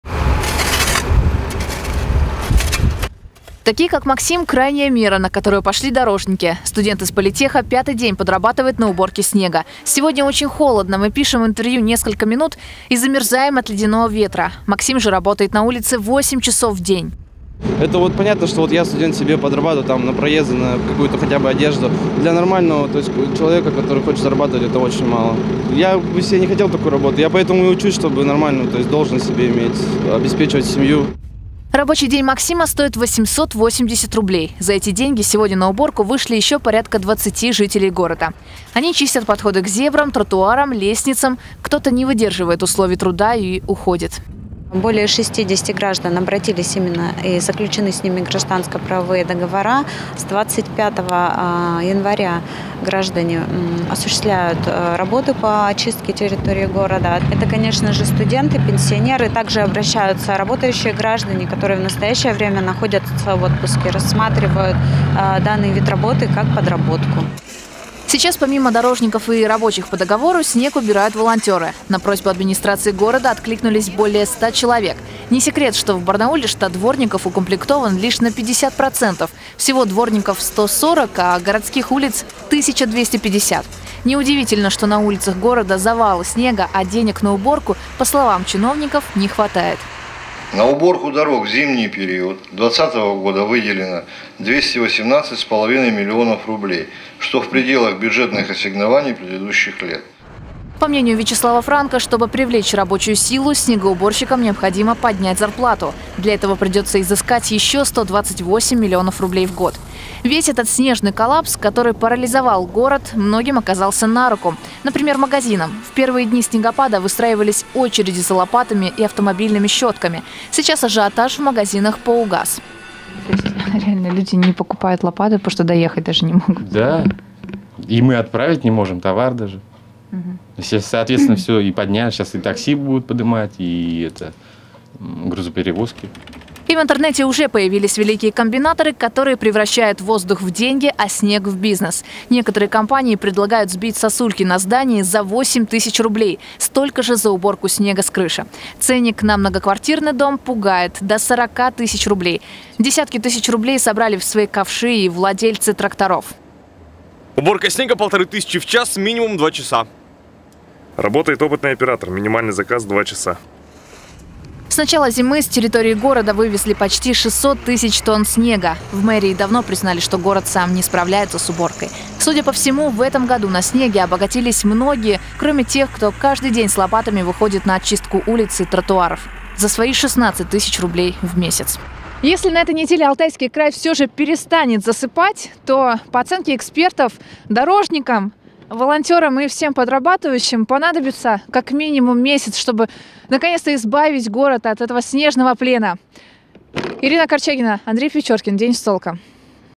Бизнес из снега: почему в Барнауле вырос спрос на дворников и кому выгодны снегопады
Сбить сосульки, вывезти снег, очистить тротуар ото льда. В этом году обильные снегопады создали десятки новых рабочих мест для жителей края. Ирина Корчагина знает, кто превратил снег в бизнес, и почему так вышло.